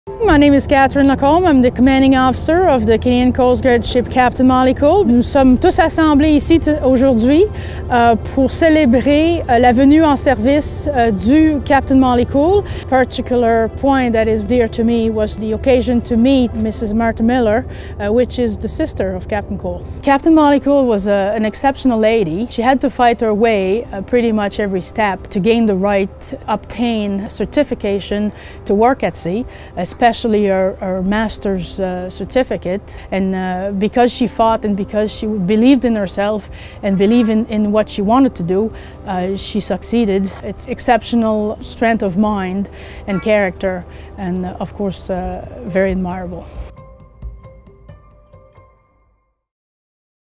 Catherine Lacombe - Remembering Captain Molly Kool
Catherine Lacombe, Commanding Officer for the Atlantic Region, is following in the footsteps of trailblazer Captain Molly Kool by empowering women and encouraging them to take on any challenge. Meeting Molly Kool's sister is definitely a day she will never forget!